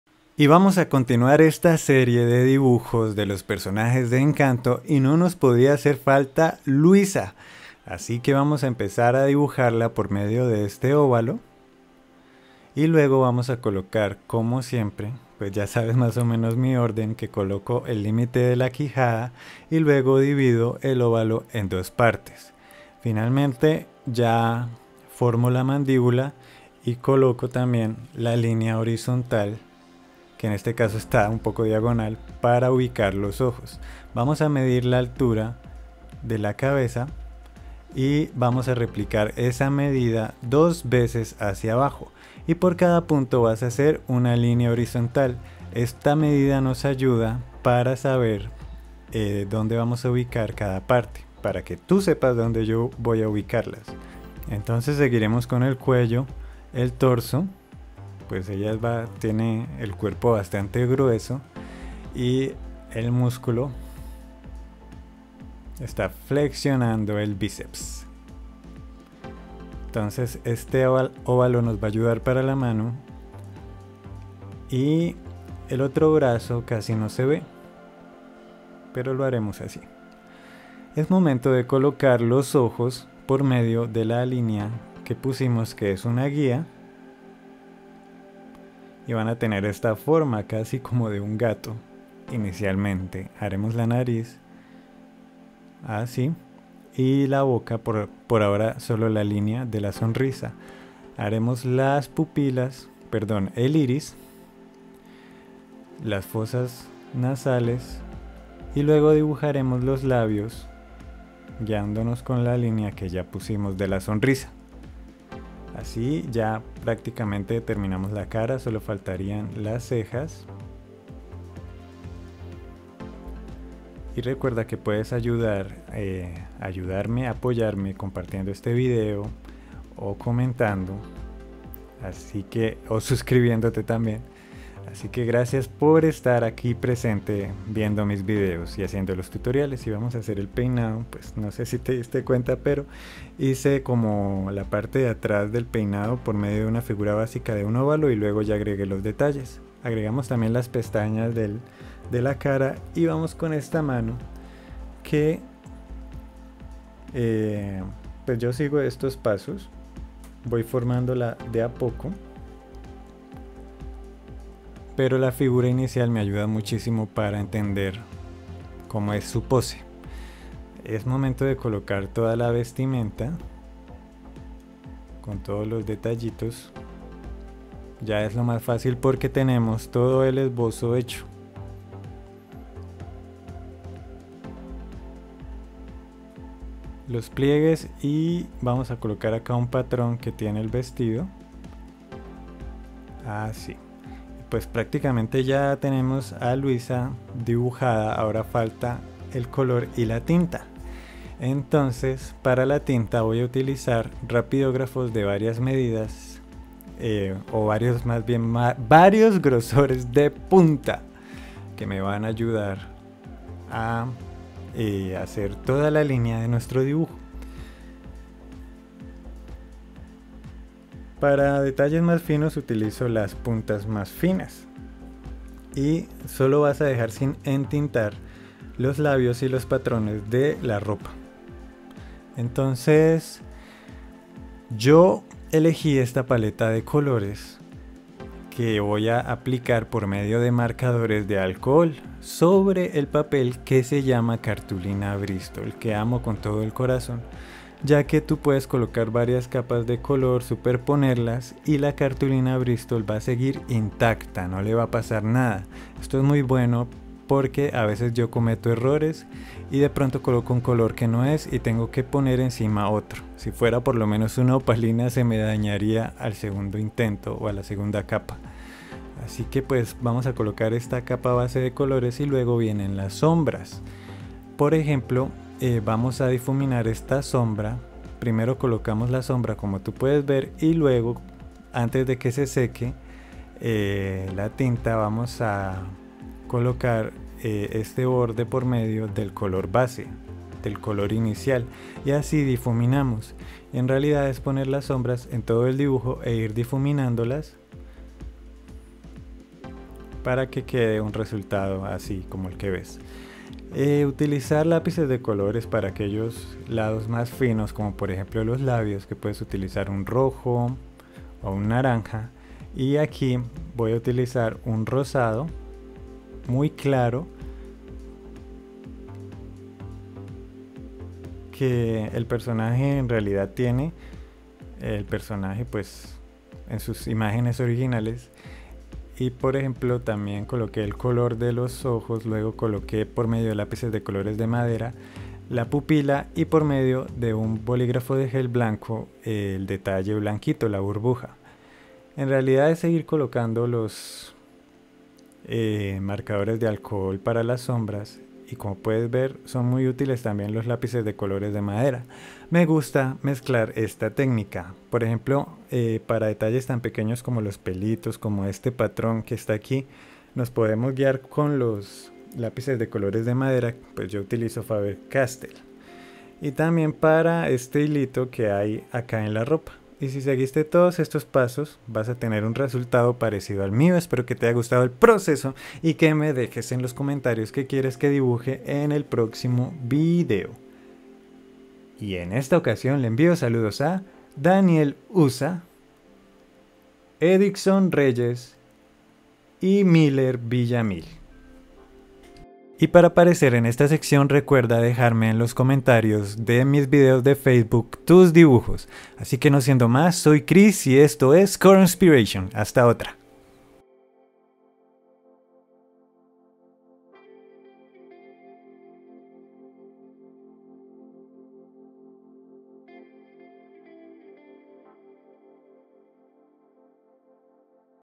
Cómo dibujar a Luisa Madrigal (Encanto) 👨‍🎨👩‍🎨Paso a paso
👉👉👉 Materiales que utilicé:
-Cartulina Bristol
-Lápiz HB para Boceto cualquier marca
-Rapidografos marca Pointer para la línea
-Lápices de Color Faber Castell
-Rotuladores Touch
-Bolígrafo de Gel blanco marca Sakura